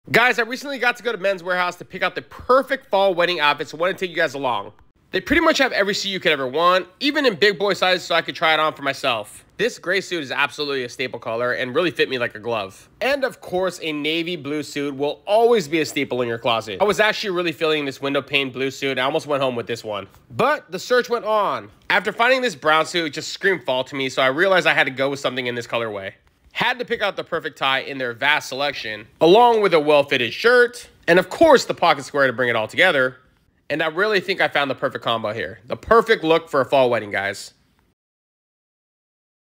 Nick Urteaga does a store walk through!!
Putting together a fall wedding look? We have you covered with everything you need and even the big sizes for us big guys!